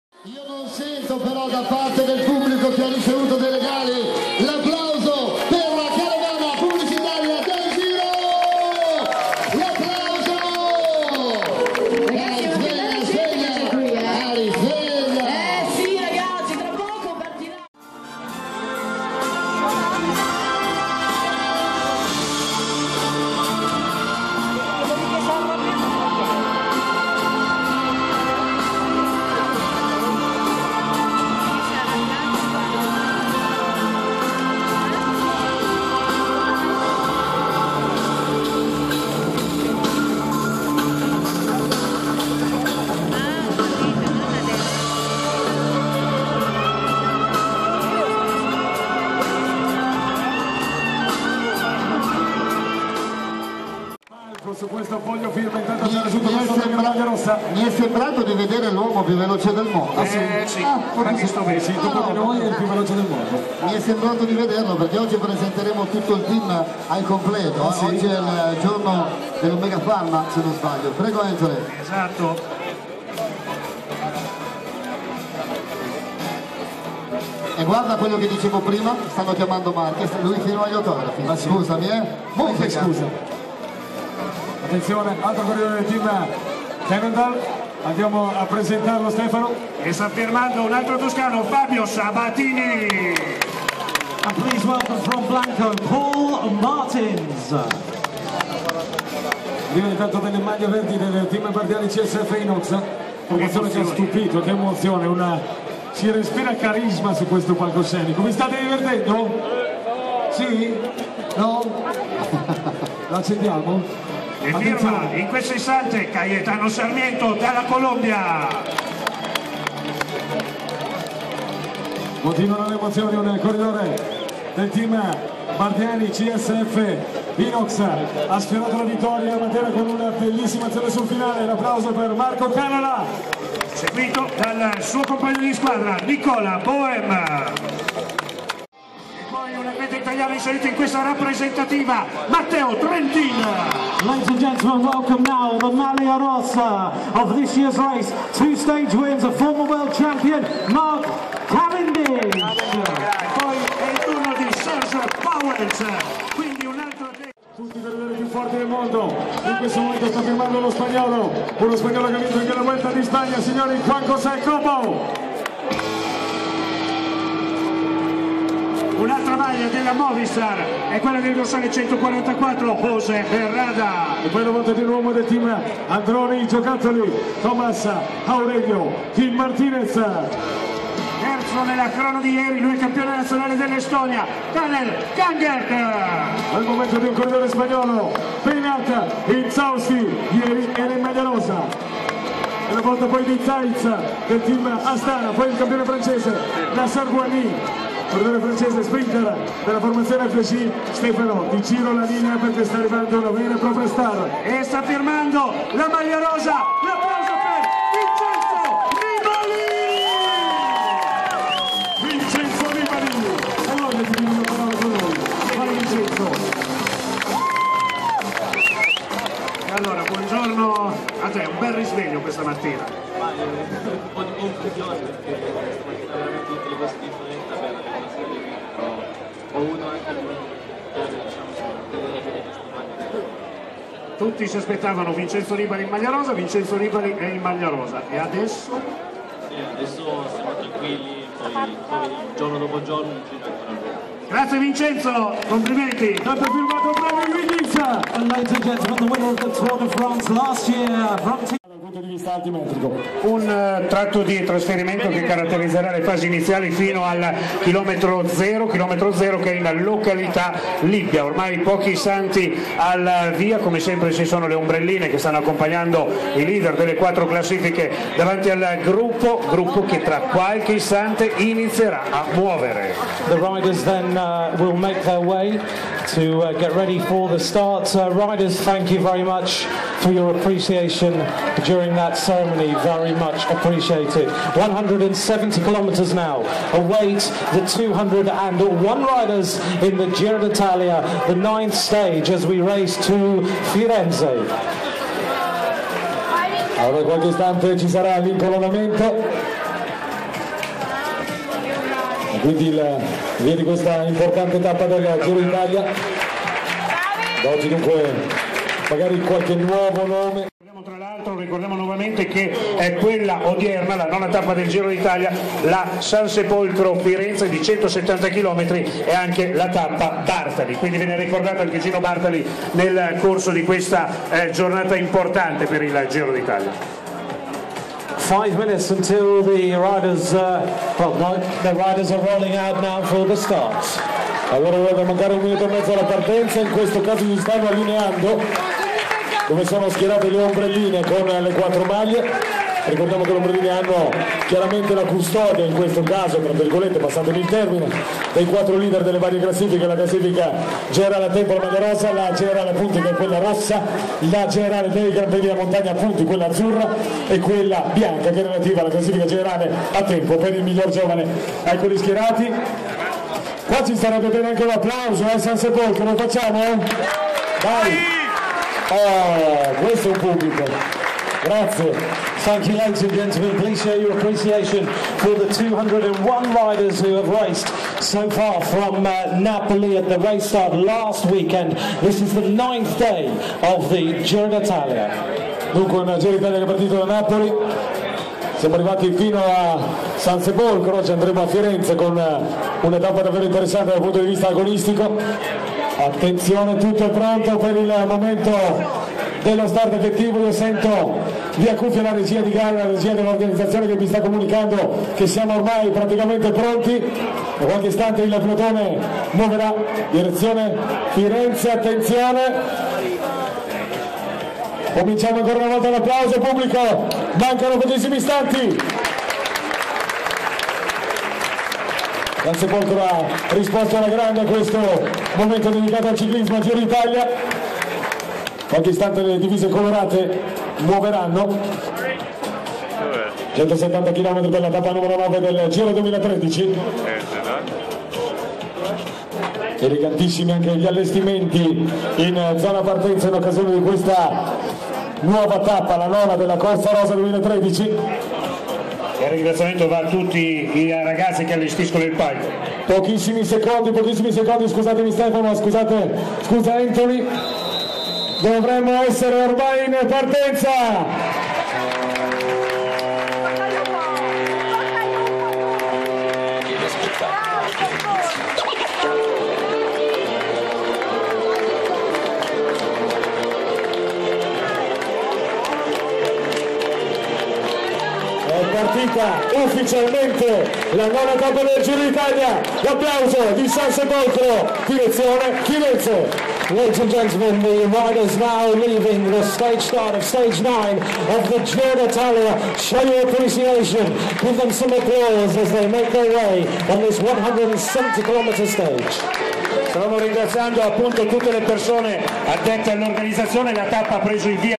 Partenza giro d'Italia san sepolcro 1013
Immagini dalla partenza della 9º tappa del Giro d'Italia 2013 da san sepolcro